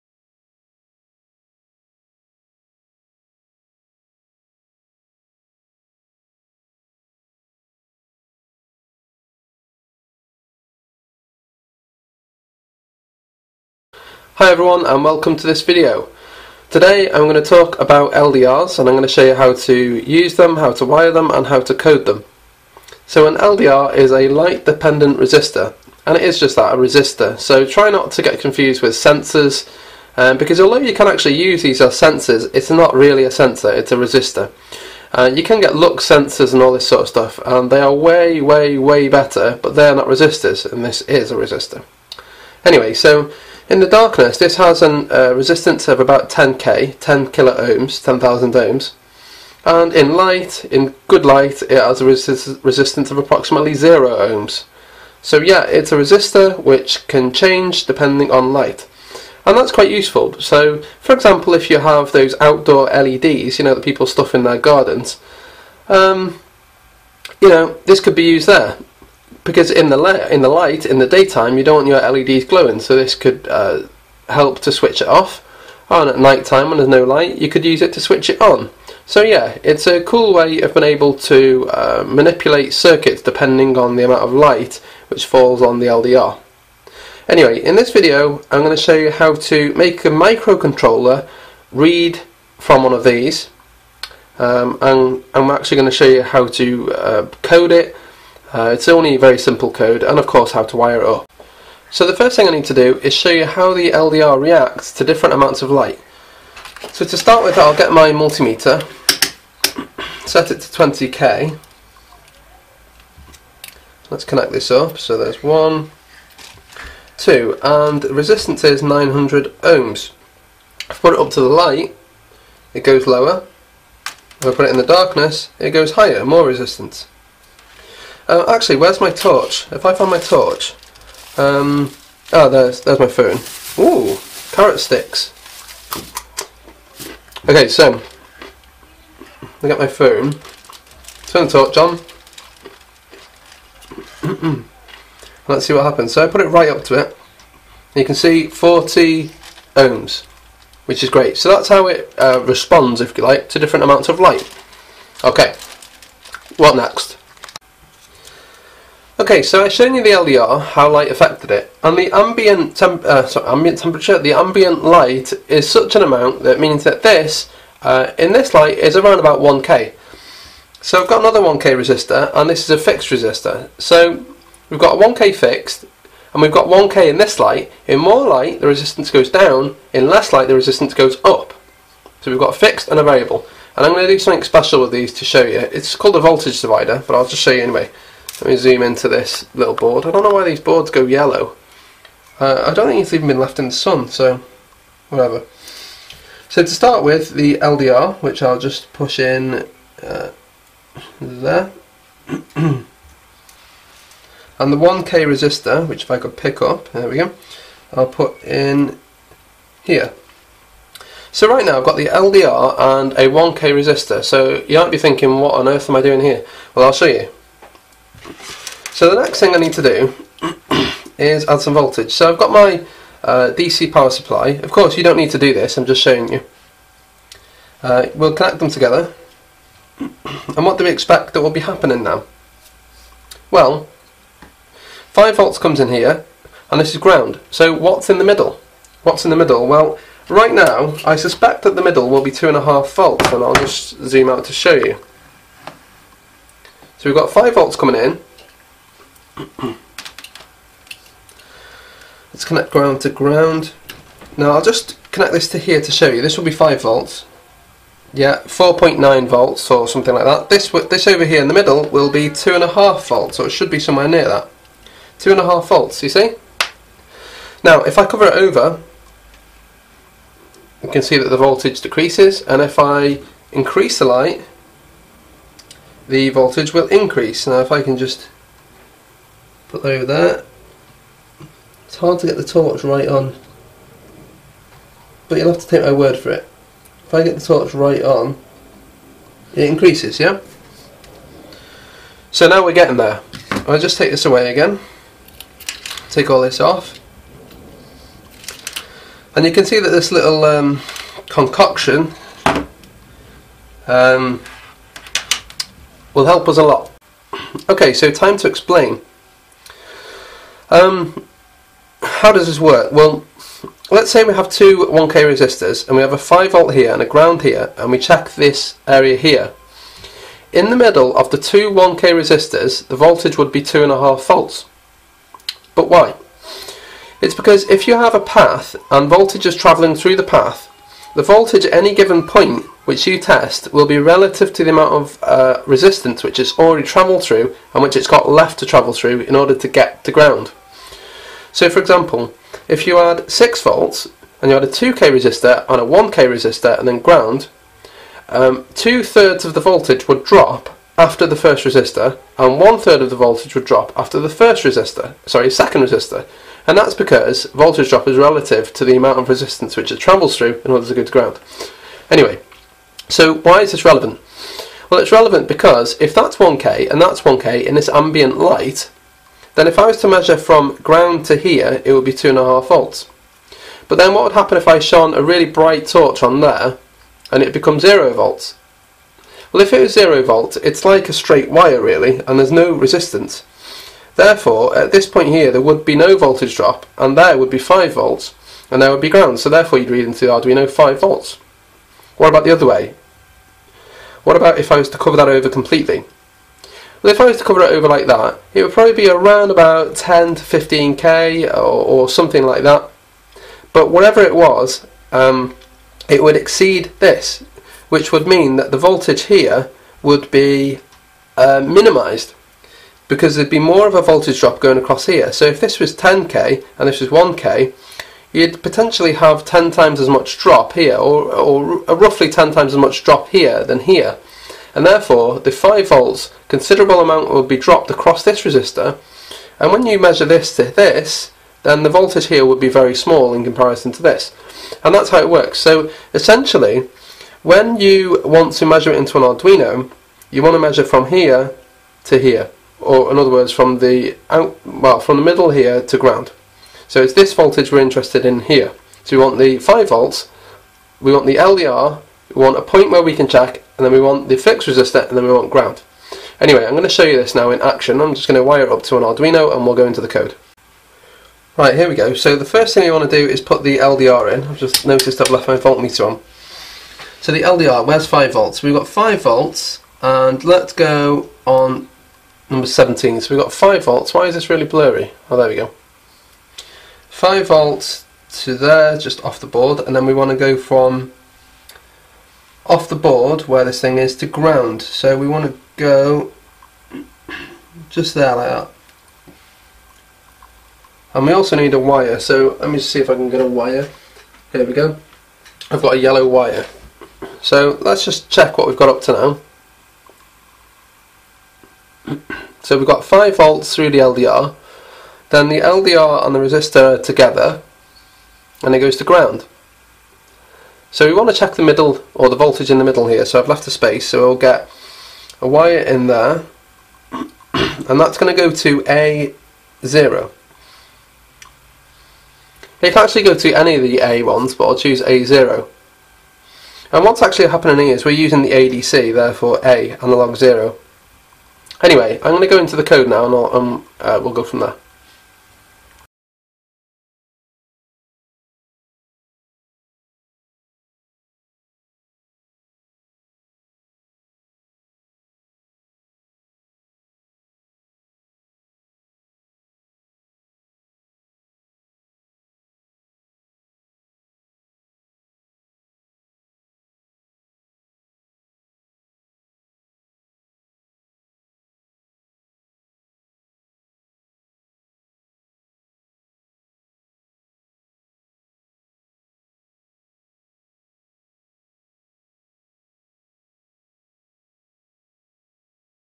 TUTORIAL: How to Wire Up, Code & Read Input From an LDR Light Dependent Resistor Arduino (Part 1)
How to work with an LDR. This part show you how to wire up the component!